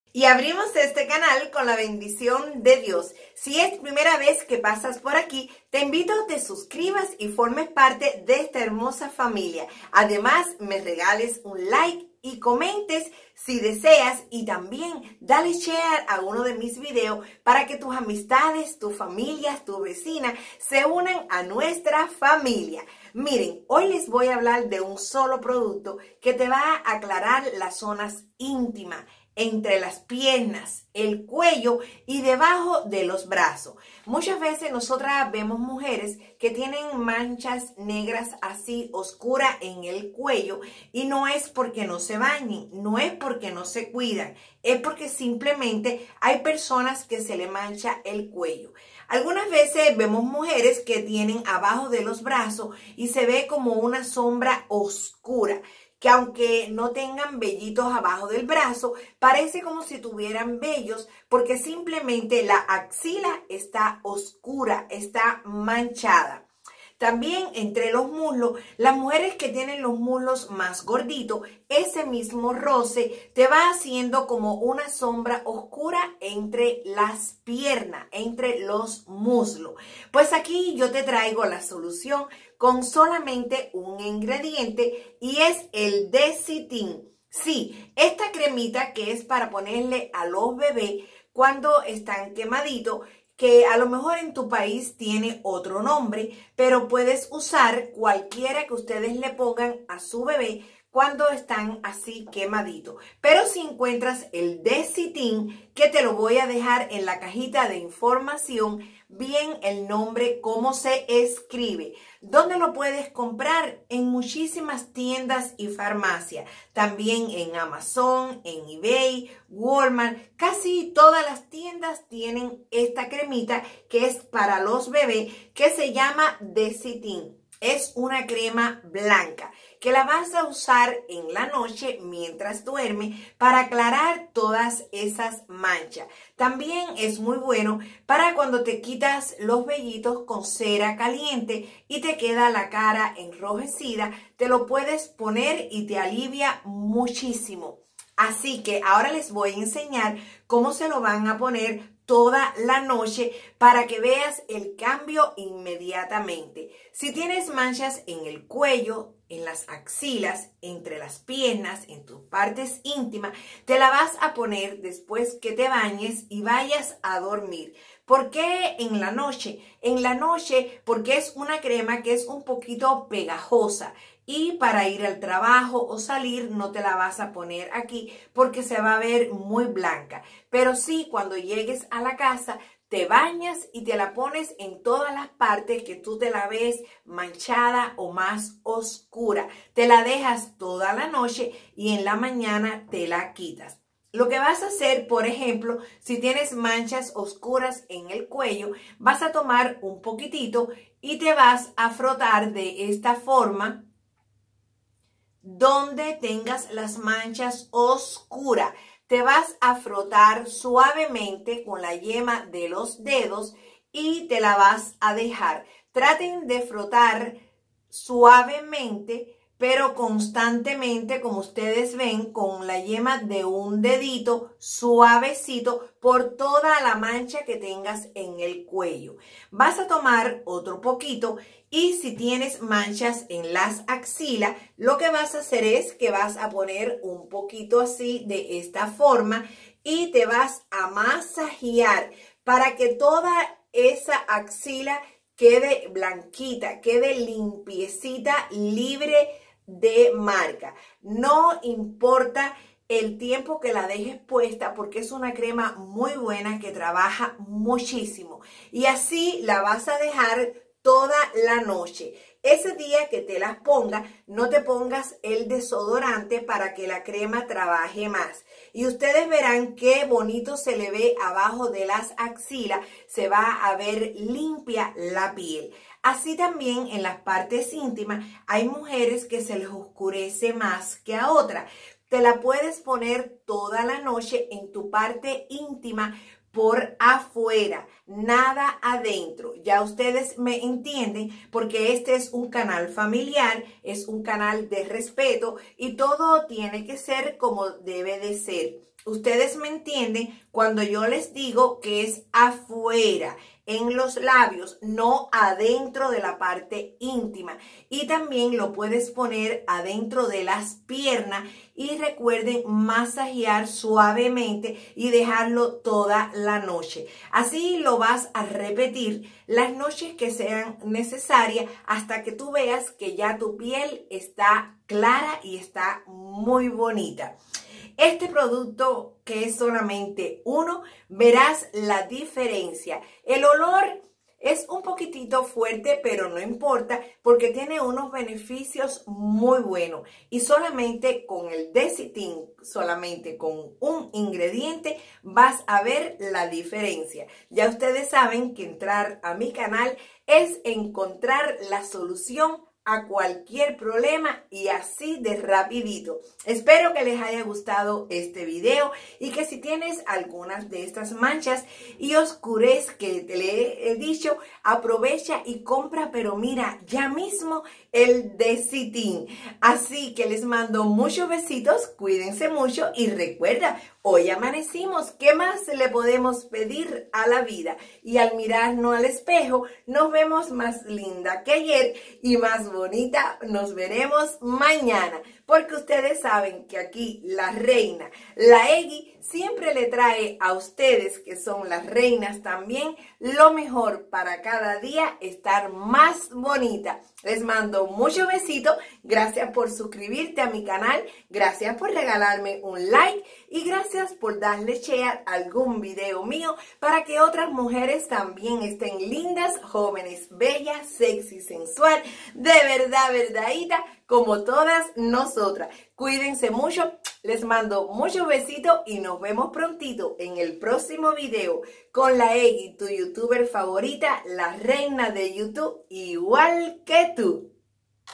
COMO QUITAR LAS MANCHAS OSCURAS DEL CUELLO + AXILAS + ZONAS INTIMAS Y ENTRE PIERNAS
LA CREMA SE LLAMA DESITIN.
BENDICIONES BESITOS